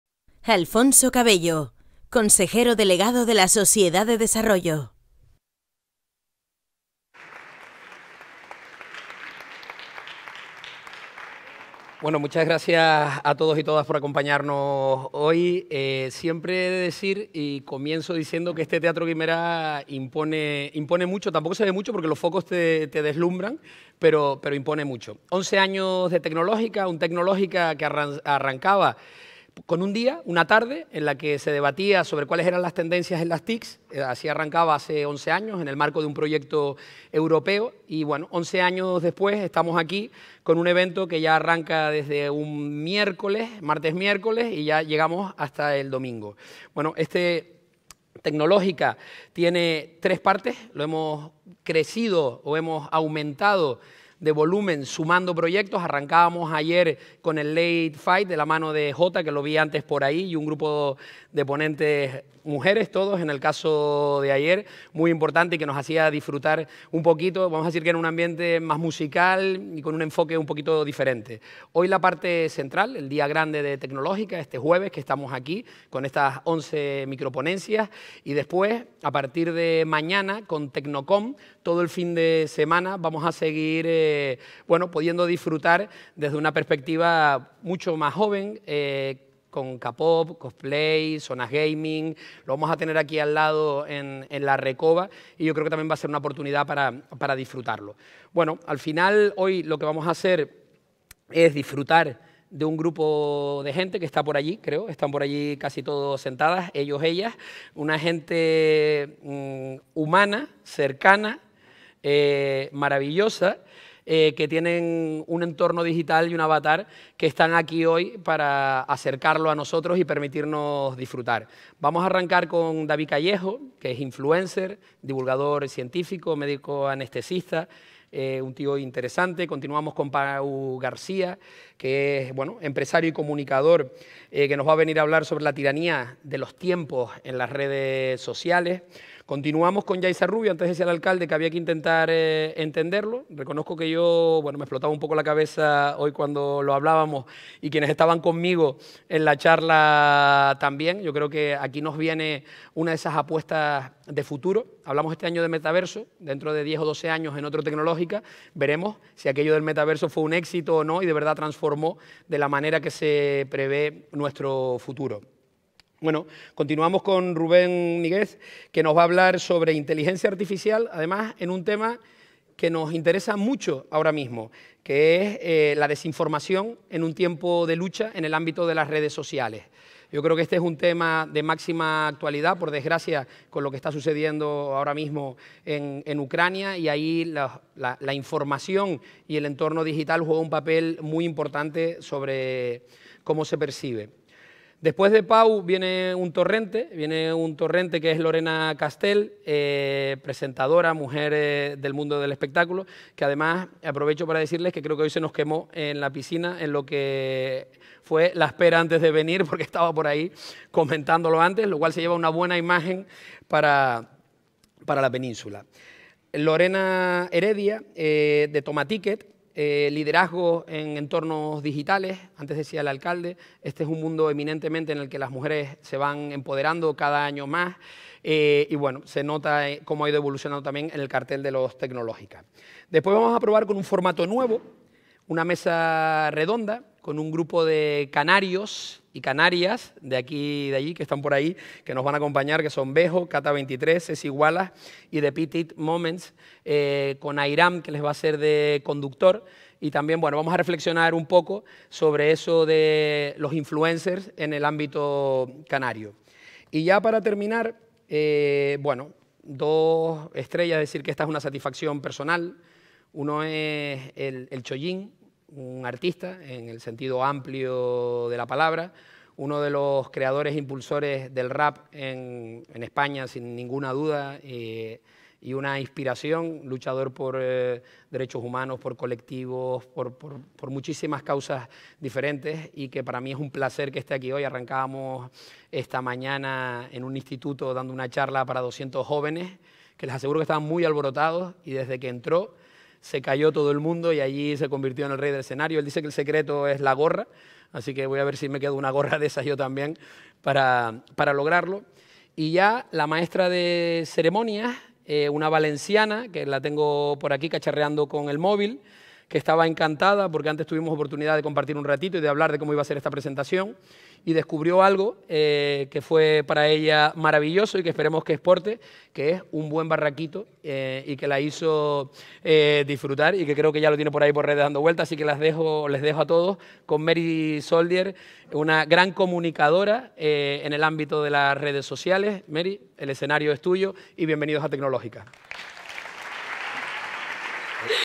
Tecnológica 22: Alfonso Cabello
Charla de Alfonso Cabello, concejal de Presidencia, ‏durante Tecnológica22, XI Encuentro de Innovación y Sociedad del Conocimiento, que tuvo lugar el 24 de marzo en Santa Cruz de Tenerife TecnológicaSC.